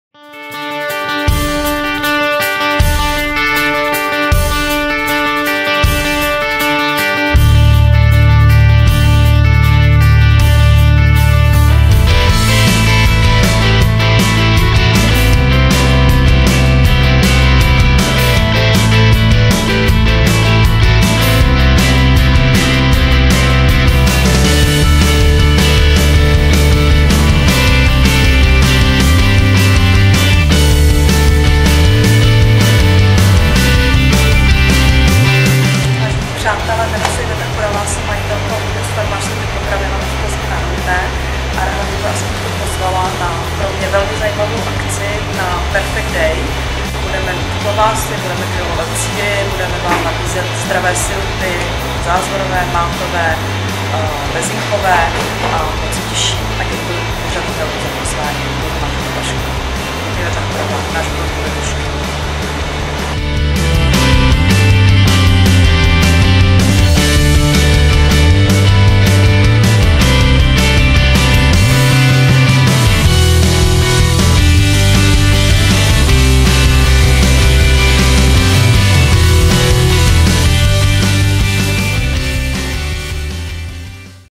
Pozvánka na Perfect Day od majitelky farmářského obchodu Náš Grunt
Další ze zajímavých hostů benefiční akce Perfect Day, jejíž celý výtěžek poputuje na pomoc dětem z dětského domova Domino!